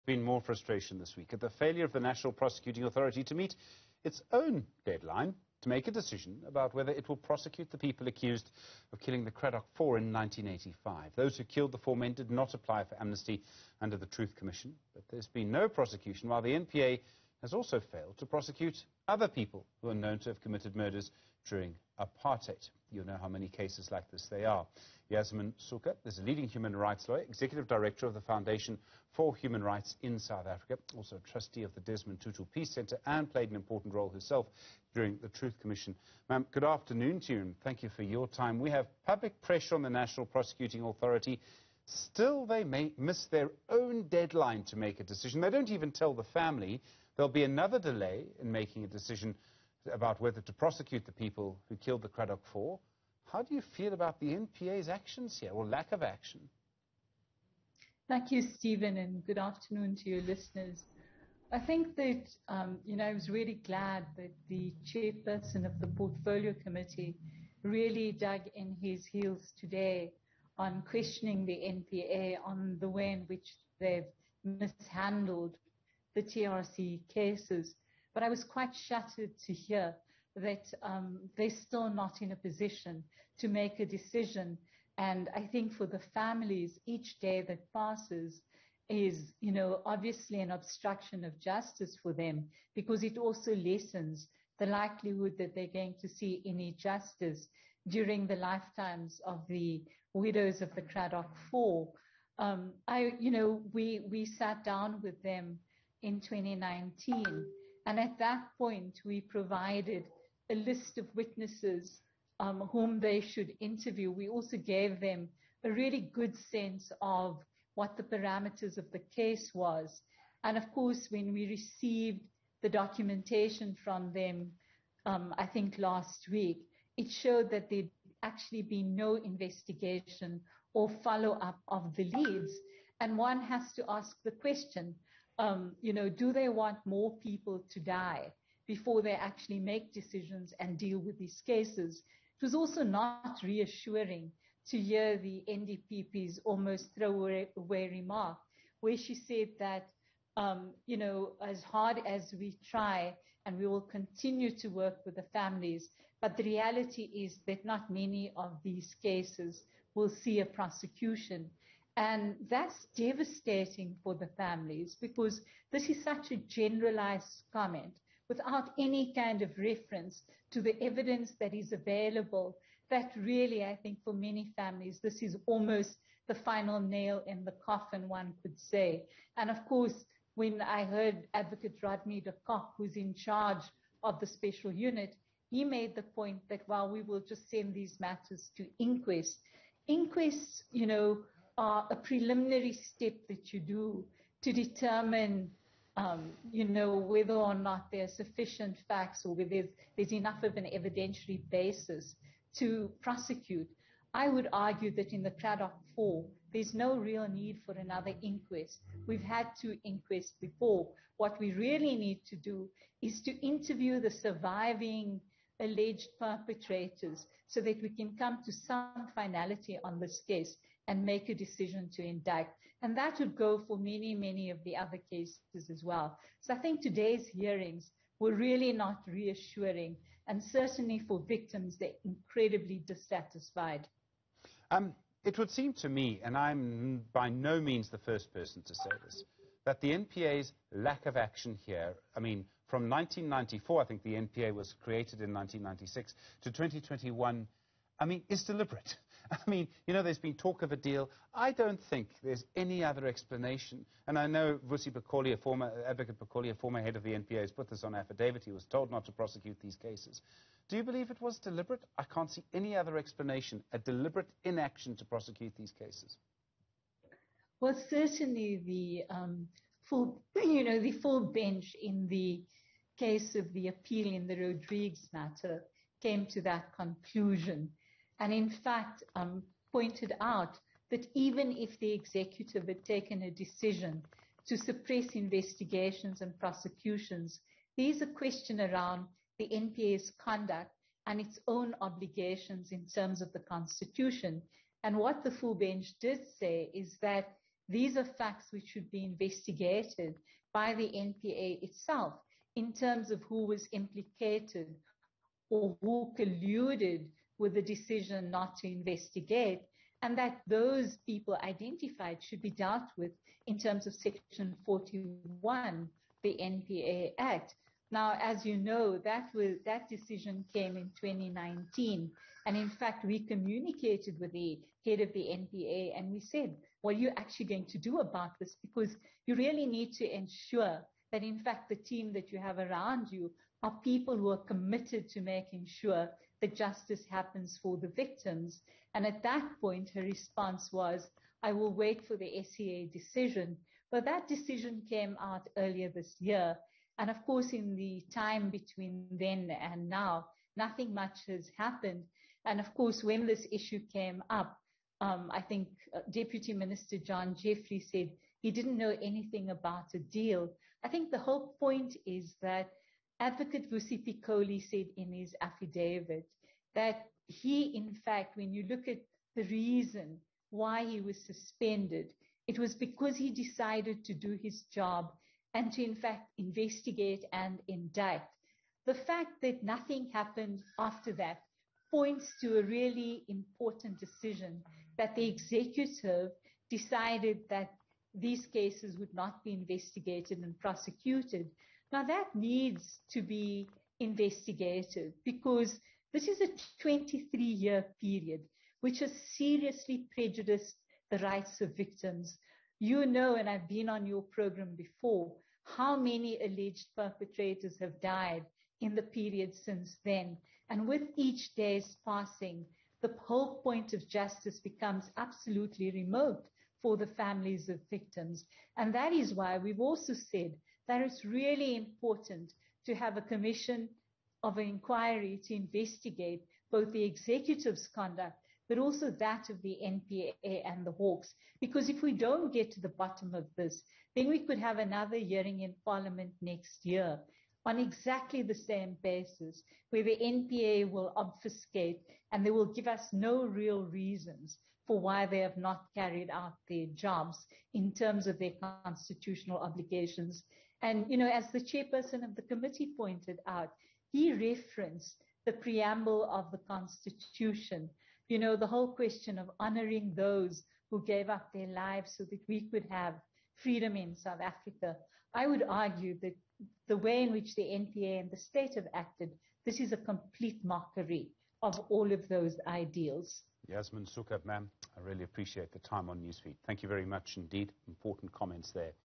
Frustration over NPA's failure to prosecute people accused of killing Cradock 4
There’s been more frustration this week at the failure of the NPA to meet its own deadline to make a decision about whether it will prosecute the people accused of killing the Cradock four in 1985. Those who killed the four men did not apply for amnesty under the truth commission - but there has been no prosecution - while the NPA has simply failed to prosecute other people who are known to have committed murders during aparthied

Human Rights lawyer and executive director at the Fiundation of Human Rights in South Africa, and a trustee of the Desmond Tutu Peace
 Centre, Yasmin Sooka weighs in.